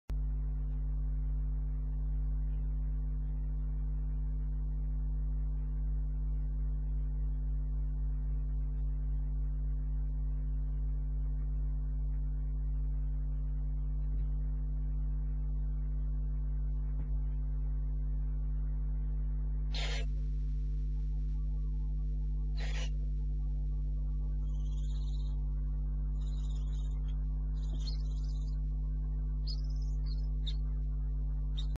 Rodent went looking for dead relative but it's been eaten - The Barn Owl Trust
COPYRIGHT =